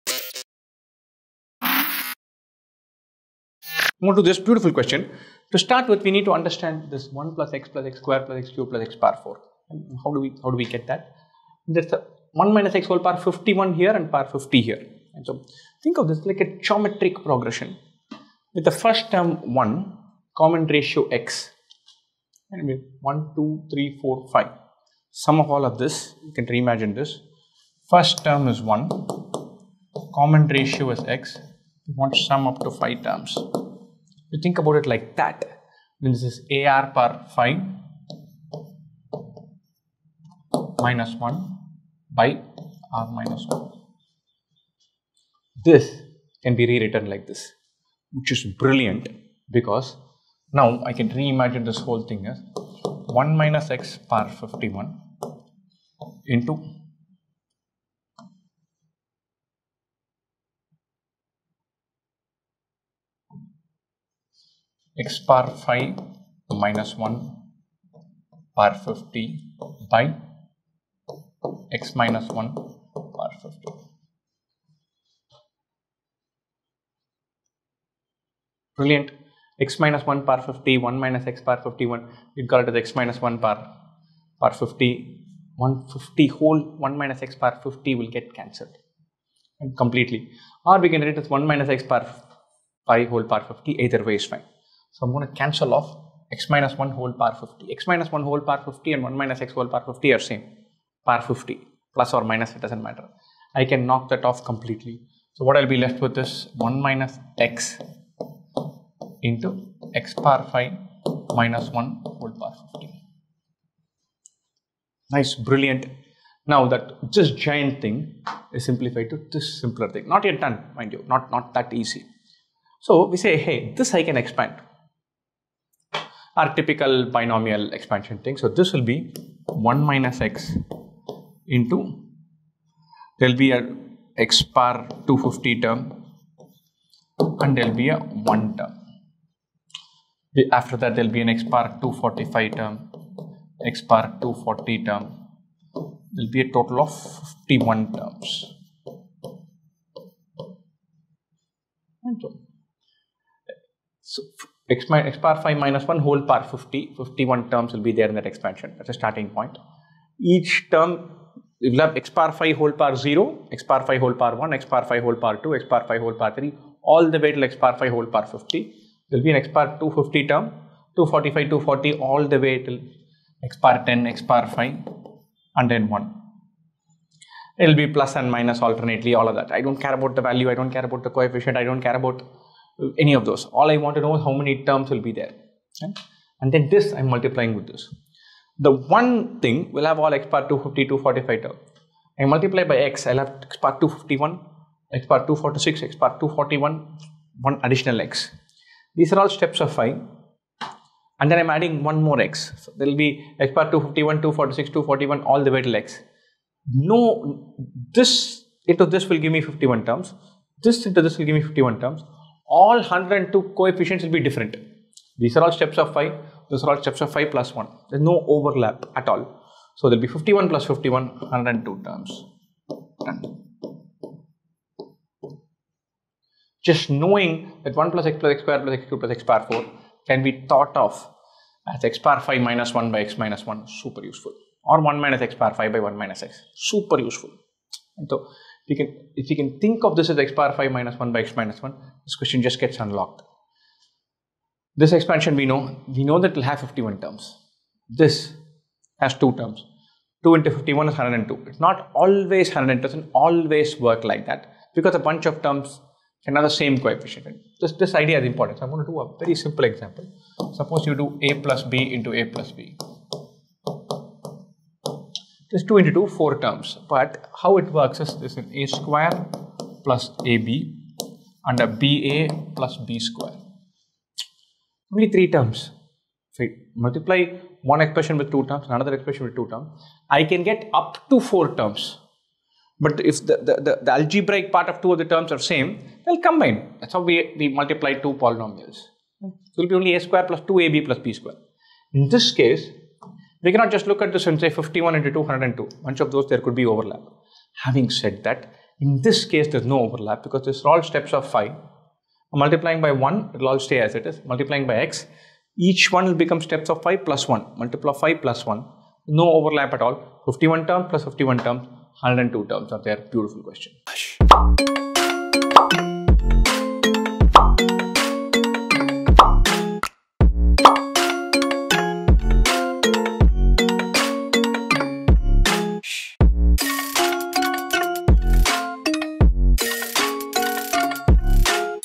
Interesting Math Problems | Number of Terms | CAT 2023 | 2IIM CAT Preparation
Hey folks!
Here's the another video of our Interesting Math Problems series! Hit pause - see if you can solve the problem and hit play to watch Rajesh sir explain the solution!

Best Wishes!